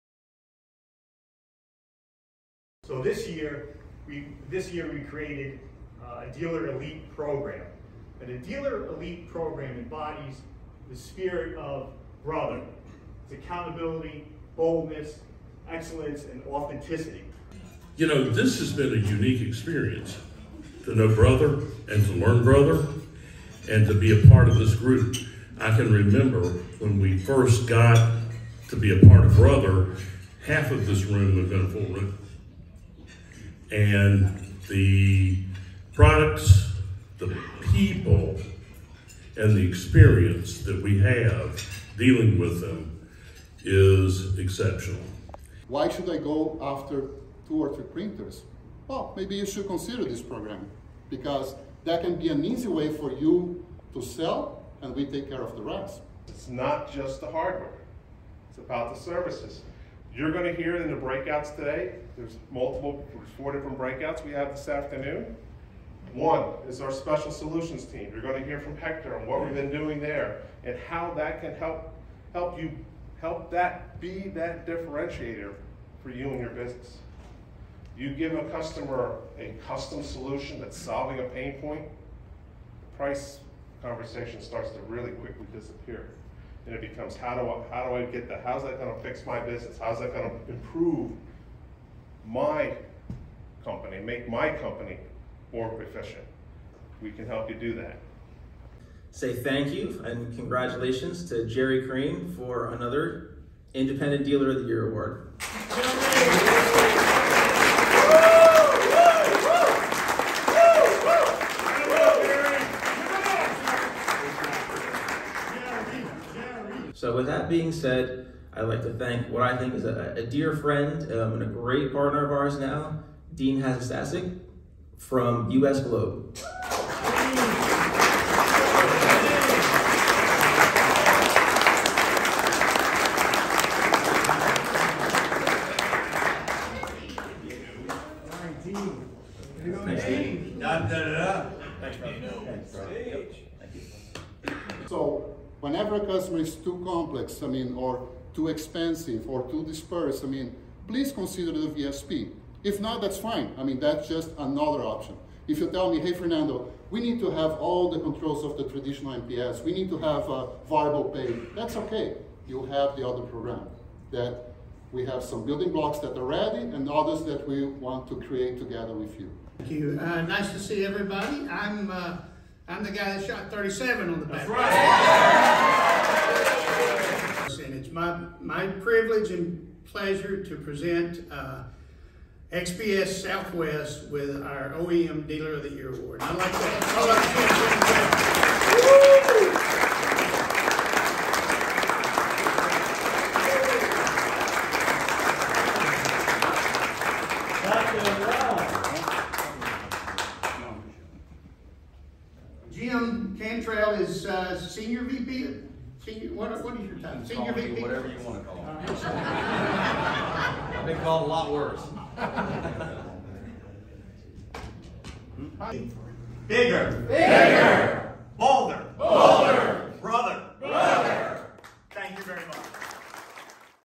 Brother Dealer Meeting Overview
Brother held their dealer meeting at Torrey Pines Lodge in CA.

Here is a video summary from their November event.

SOURCE Industry Analysts Inc.